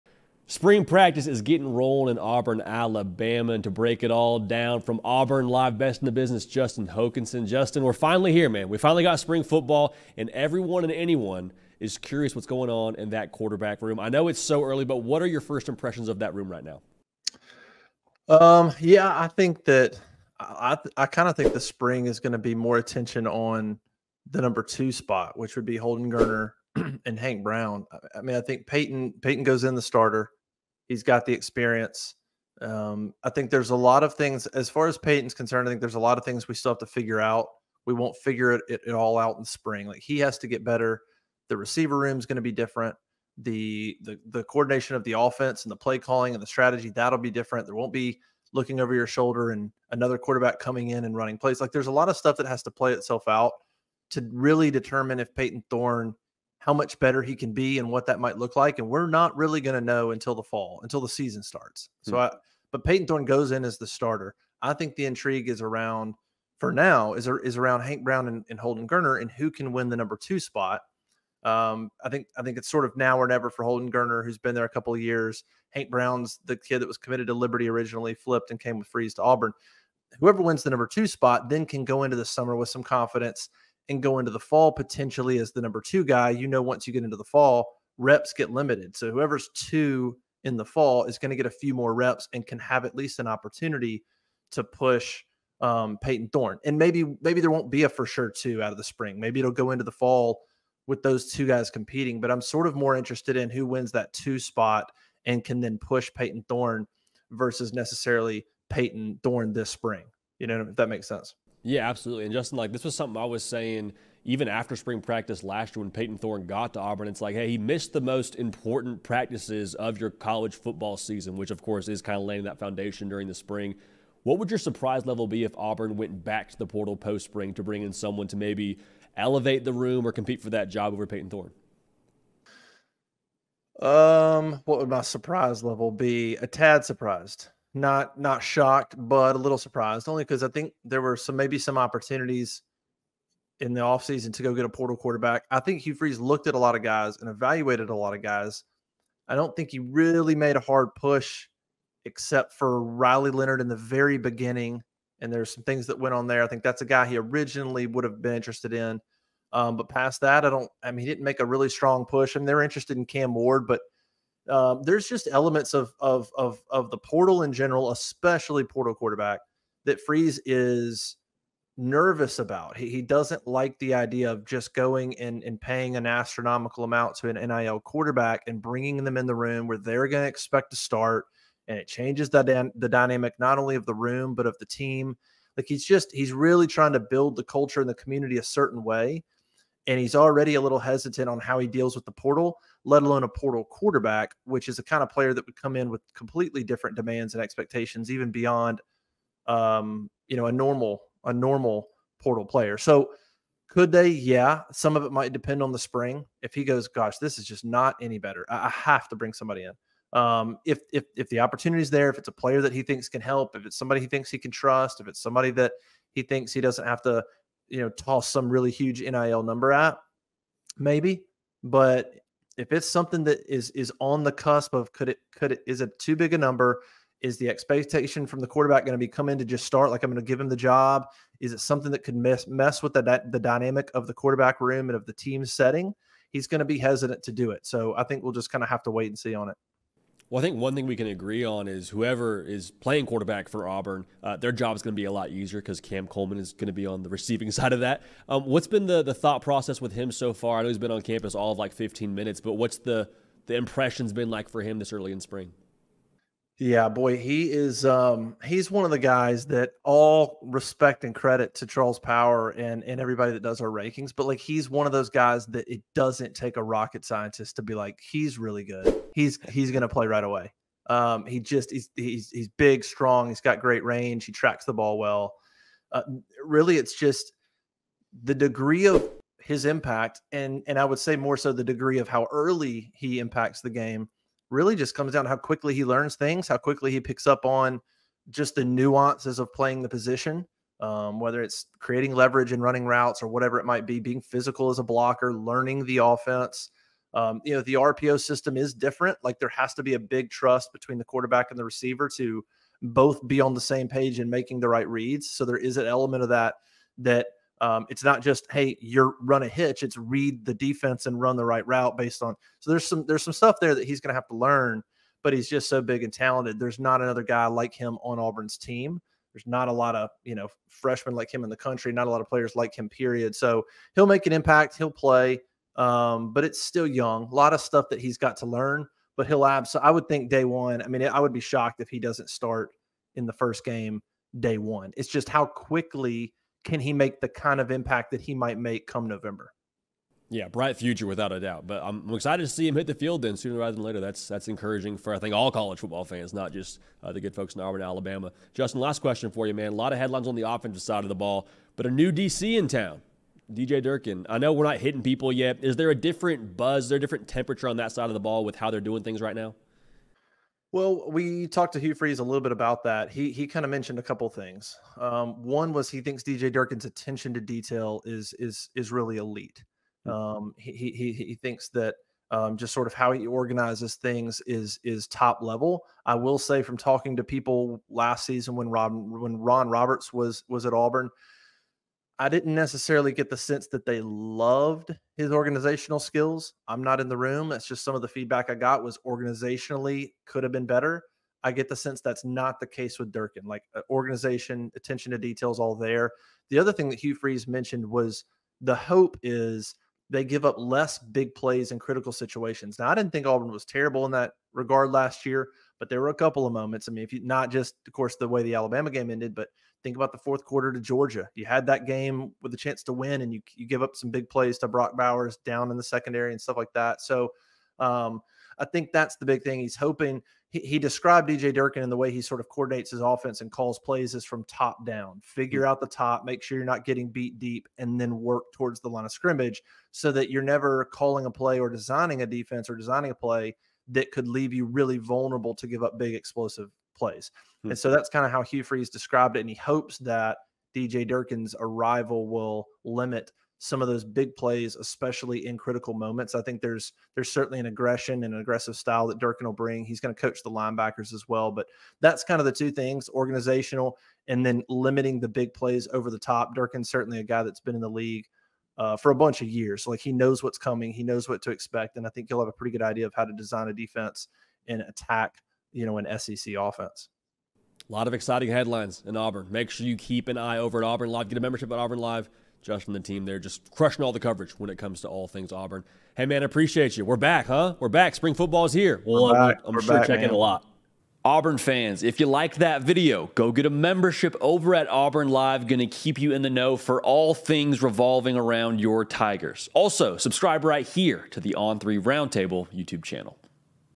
Will Auburn Tigers Add Transfer Portal QB Post Spring? Cam Coleman 1st Impressions? Spring Ball Buzz
The Auburn Tigers are underway in spring ball ahead of the 2024 season. Justin Hokanson of Auburn Live joins the On3 Roundtable to discuss the big talking points ahead of year two in the Hugh Freeze era.